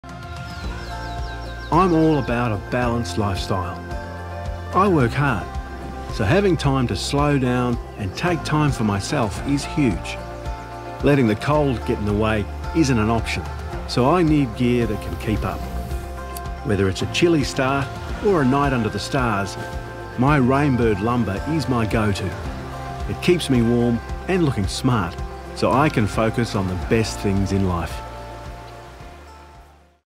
Rainbird Lumber Jacket
At Rainbird we know that keeping warm is key to your everyday essential needs and designing premium outerwear for your protection is our purpose. That's why we trust in our best selling fleece Lumber Jacket. We've designed this winter hero with a 450gsm tek high pile sherpa fleece, perfect for braving the cold, and bonded it to a soft velvet outer fleece that is not only hard working but made to last.